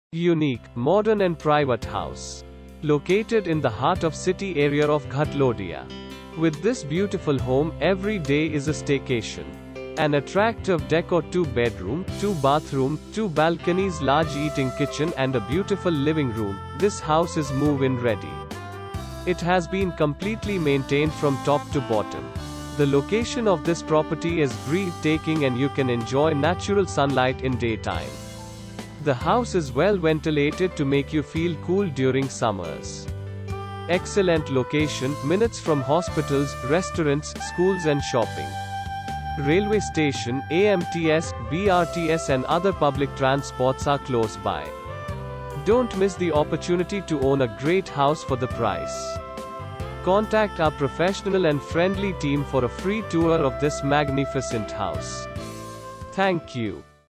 Ratnadeep Tower , Ghatlodiya
Unique, modern and private house. Located in the heart of city area of Ghatlodiya. With this beautiful home, every day is a staycation. An attractive decor two Bedroom, two bathroom, two Balconies large eating kitchen and a beautiful living room, this house is move-in ready! It has been completely maintained from top to bottom. The location of this property is breathe taking and you can enjoy natural sunlight in day time. The house is well ventilated to make you feel cool during summers. Excellent location, minutes from hospitals, restaurants, schools and shopping. Railway Station, AMTS, BRTS and other public transports are close by. Don't miss the opportunity to own a great house for the price! Contact our professional and friendly team for a free tour of this magnificent house! Thank You.